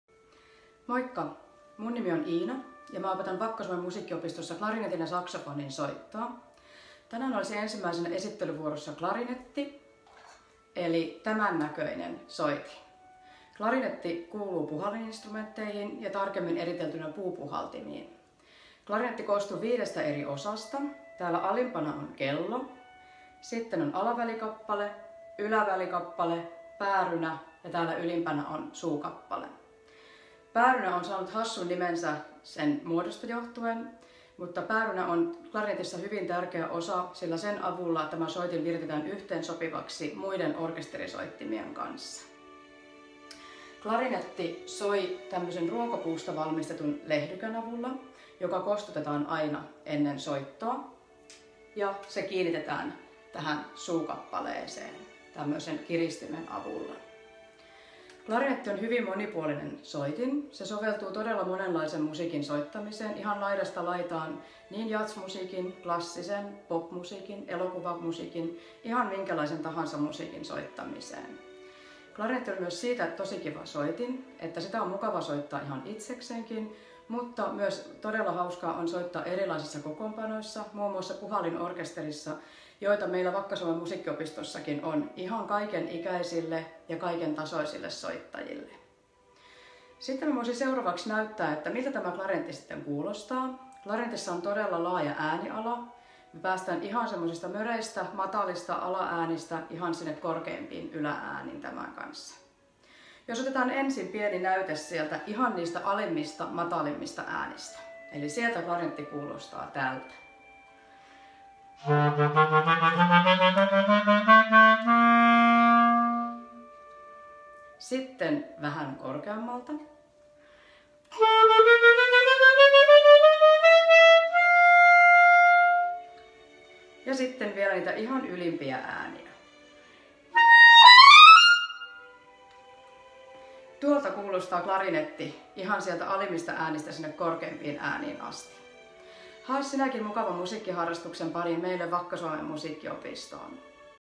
Klarinetti - puupuhaltimet esittelyssä
Musiikkiopistomme opettaja Iina Loponen esittelee klarinettia.

Ilmoittautuminen Vakka-Suomen musiikkiopistoon aukeaa 4.5.2020 klo 18! Tervetuloa!